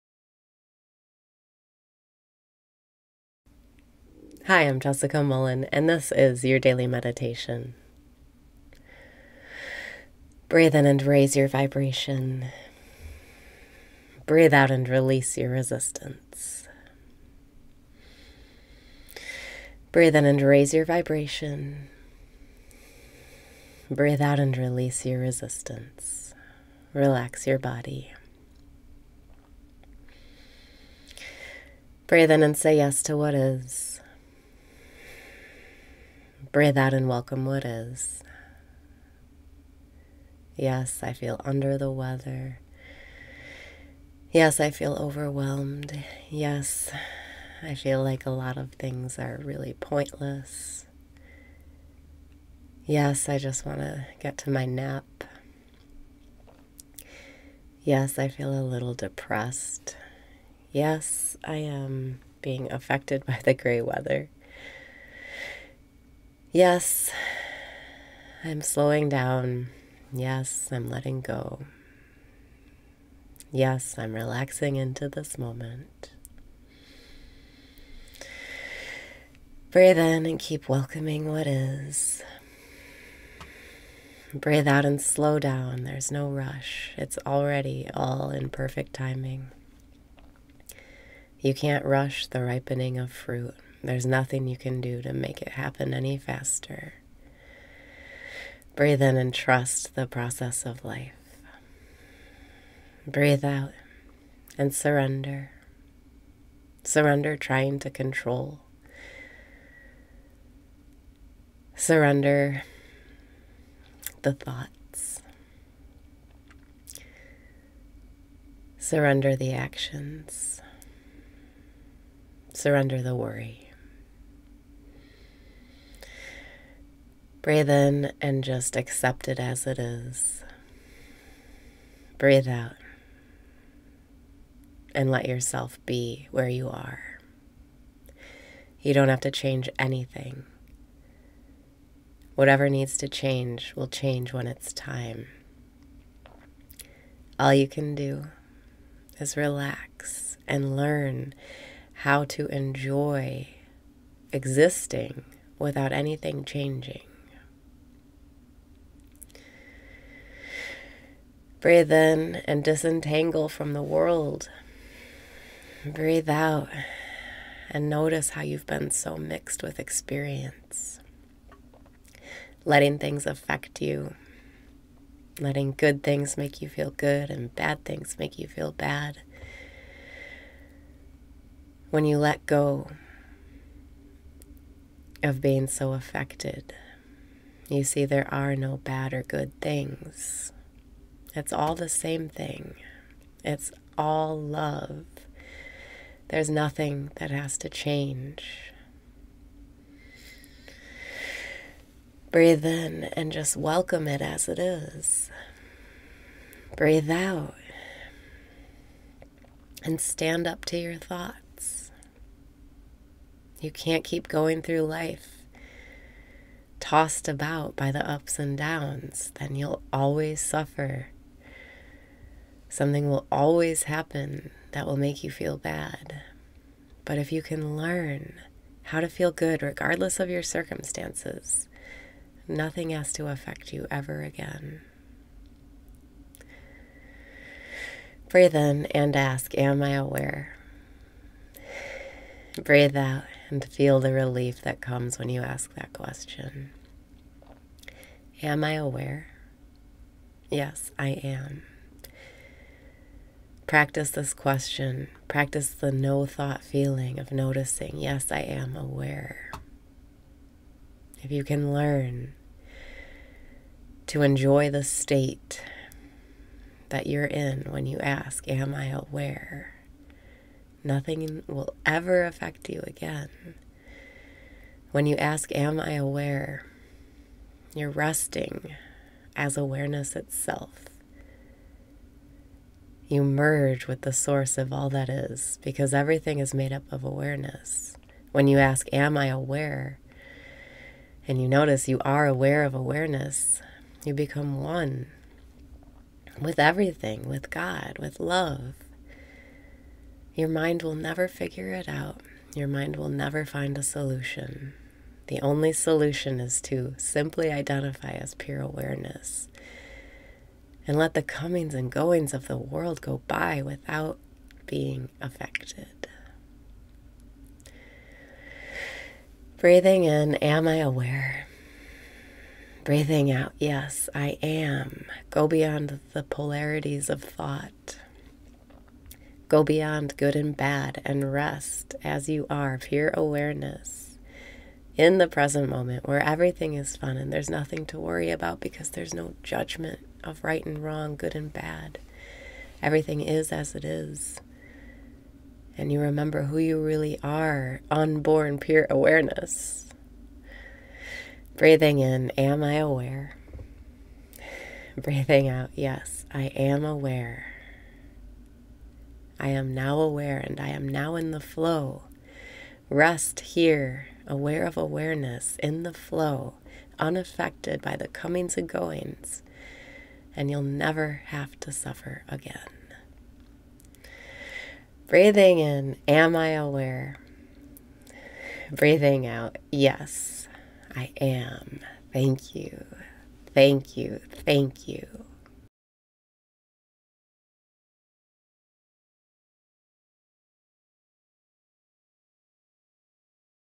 Daily Meditation - Enjoy Existing Without Anything Changing
+TRANSCRIPT+

Yes, I feel under the weather. Yes, I feel overwhelmed. Yes, I feel like a lot of things are really pointless. Yes, I just want to get to my nap. Yes, I feel a little depressed. Yes, I am being affected by the grey weather. Yes, I'm slowing down. Yes, I'm letting go. Yes, I'm relaxing into this moment.

Breathe in and keep welcoming what is.
Breathe out and slow down.

There's no rush. It's already all in perfect timing. You can't rush the ripening of fruit. There's nothing you can do to make it happen any faster.

Surrender trying to control. Surrender the thoughts. Surrender the actions. Surrender the worry.

Breathe in and just accept it as it is.
Breathe out and let yourself be where you are.

You don't have to change anything. Whatever needs to change will change when it's time. All you can do is relax and learn how to enjoy existing without anything changing.

Breathe in and disentangle from the world.
Breathe out and notice how you've been so mixed with experience.

Letting things affect you. Letting good things make you feel good and bad things make you feel bad. When you let go of being so affected, you see there are no bad or good things. It's all the same thing. It's all love. There's nothing that has to change.

Breathe in and just welcome it as it is.
Breathe out and stand up to your thoughts.

You can't keep going through life tossed about by the ups and downs. Then you'll always suffer. Something will always happen that will make you feel bad. But if you can learn how to feel good regardless of your circumstances, nothing has to affect you ever again.

Breathe out and feel the relief that comes when you ask that question.

Am I aware? Yes, I am. Practice this question. Practice the no thought feeling of noticing, yes, I am aware. If you can learn to enjoy the state that you're in when you ask, am I aware? Nothing will ever affect you again. When you ask, am I aware? You're resting as awareness itself. You merge with the source of all that is because everything is made up of awareness. When you ask, am I aware? And you notice you are aware of awareness, you become one with everything, with god, with love.

Your mind will never figure it out. Your mind will never find a solution. The only solution is to simply identify as pure awareness and let the comings and goings of the world go by without being affected.

Go beyond the polarities of thought. Go beyond good and bad and rest as you are, pure awareness, in the present moment where everything is fun and there's nothing to worry about because there's no judgment of right and wrong, good and bad. Everything is as it is. And you remember who you really are, unborn, pure awareness.

I am now aware and I am now in the flow. Rest here, aware of awareness in the flow, unaffected by the comings and goings, and you'll never have to suffer again.